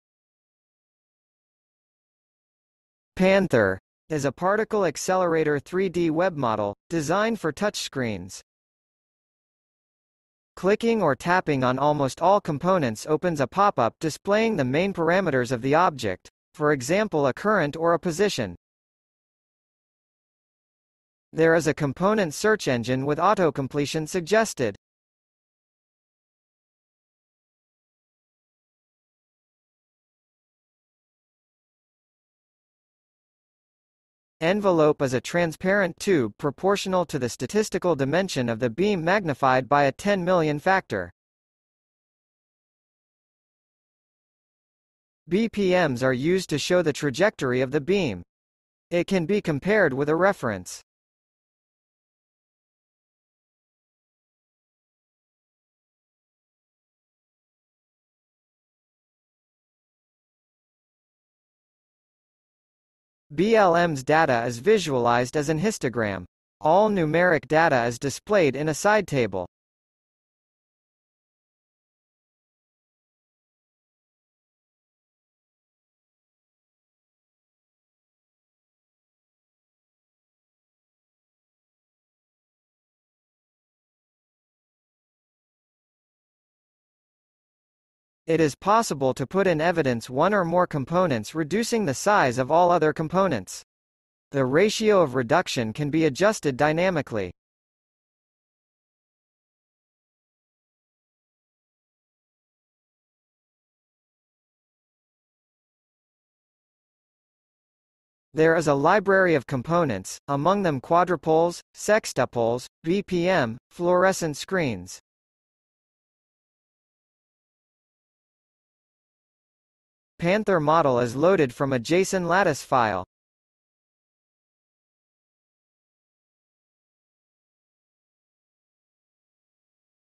PAnTHer
PAnTHer is a 3D model of a Particle Accelerator
BLM values can be shown as an histogram.
BPM values  can be shown as a trajectory.
Beam size simulations can be shown as a transparent envelope.

(numeric data in demo are not in real time)

Edited by Andrea Apollonio